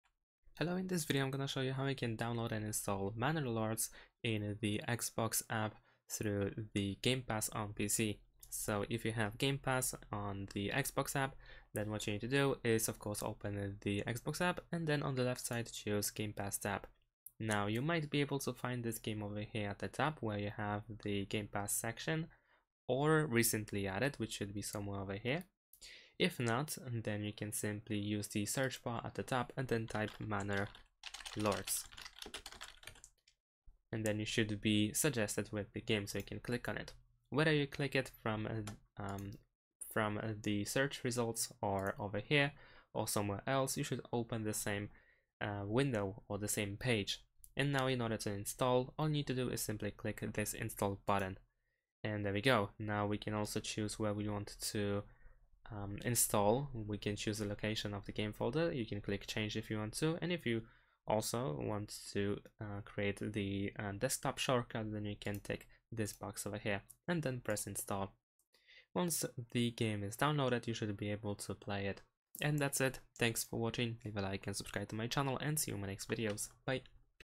How to Download Manor Lords in Xbox App with Game Pass on PC? Play Manor Lords with Xbox Game Pass
Explore the process of downloading Manor Lords through the Xbox app on PC with Xbox Game Pass, enabling you to access and enjoy the game with your subscription.